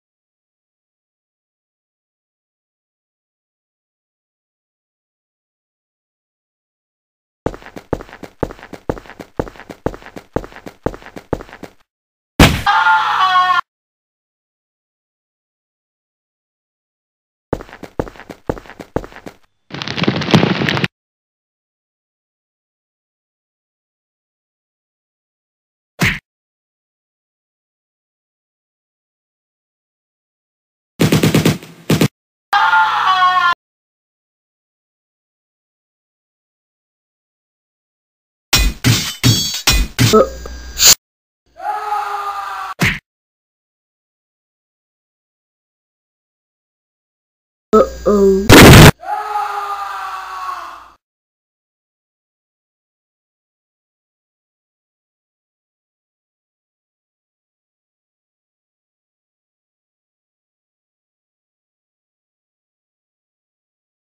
LEGO - Violence tests
this is my very first time making this kind of animation so i wanted to prove it.